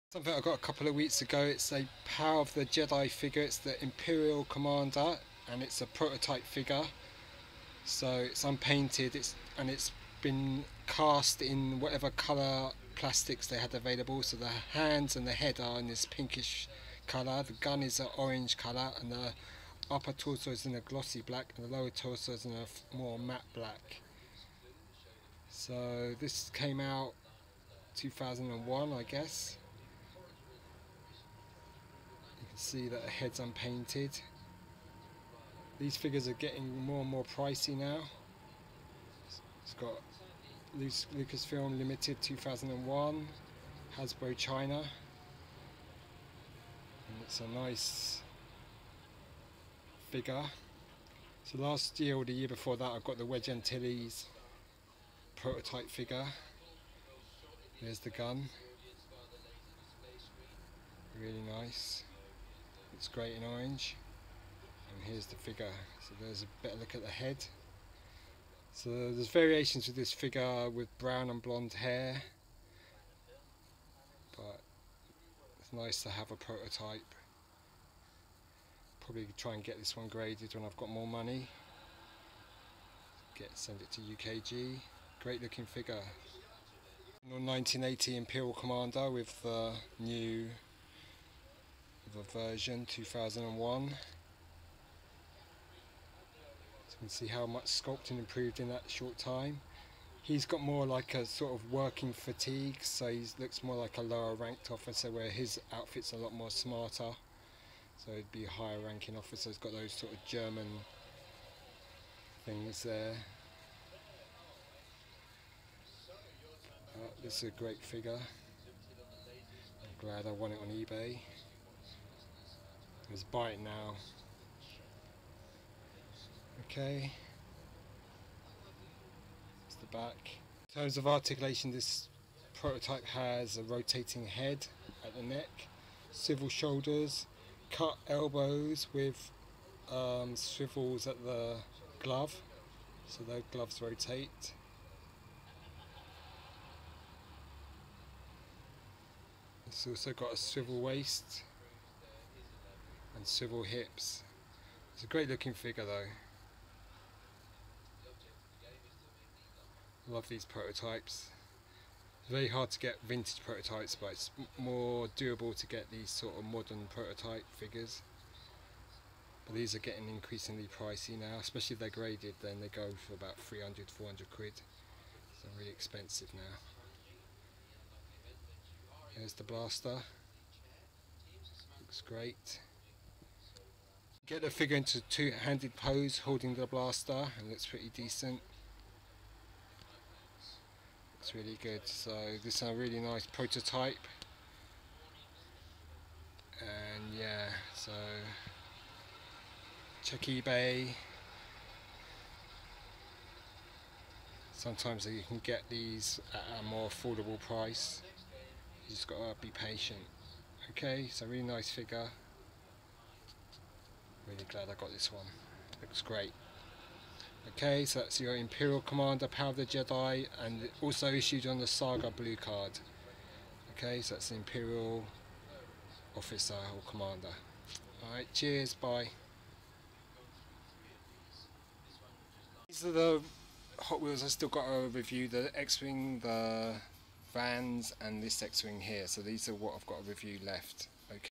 POTJ 2001 Imperial Officer action figure prototype/ test shot
I got this a couple of weeks ago at a good price! These figures are getting pricey now!